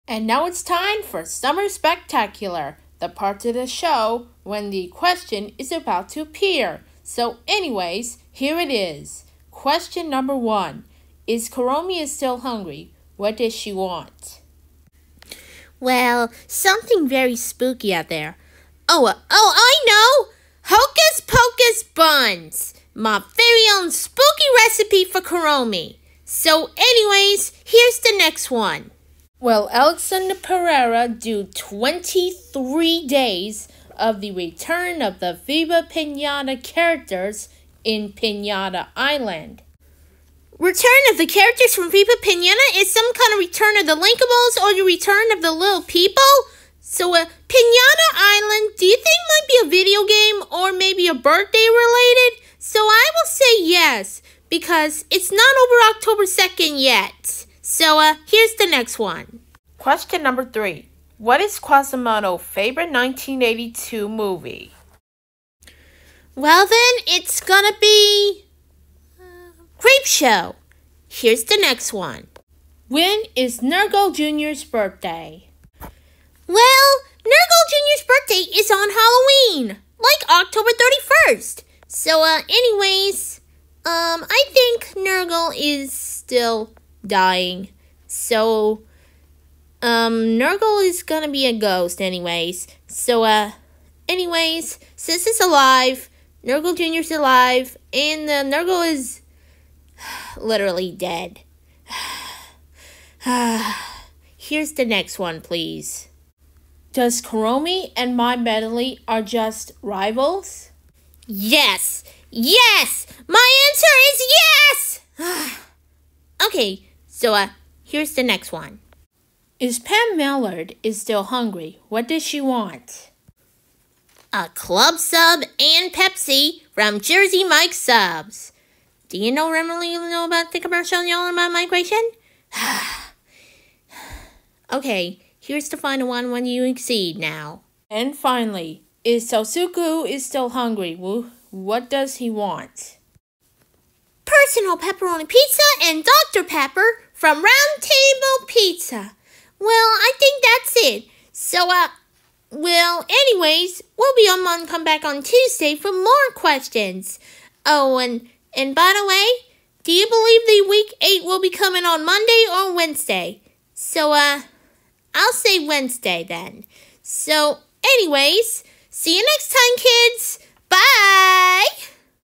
What’s Up Deena Kidoonia #398-404
Question #1 Answer: Kuromi wants to get Hocus Pocus Buns. Can’t wait for Halloweentoono 5.

Question #2 Answer: Pinata Island, huh? My answer is yes.

Question #3 Answer: Quasimodo’s favorite movie in 1982 is Creepshow.

Question #4 Answer: Nergal Jr.’s birthday is on October 31st, the same day of Scarm Scaremly’s birthday, Kuromi’s birthday and Halloween. But seriously, do you think Nergal might be dead or something?

Question #5 Answer: My answer is yes because Kuromi and My Melody are rivals.

Question #6 Answer: Pam Mallard wants to get a club sub and Pepsi from Jersey Mike’s Subs.

Question #7 Answer: Sosuke wants to get a personal pepperoni pizza and Dr. Pepper from Round Table Pizza.

Requested by: Kristian TV 2024 Cartoon Cartoons, Quasimodo, Elias Martinez, Alexander Pereira and Petey Hernandez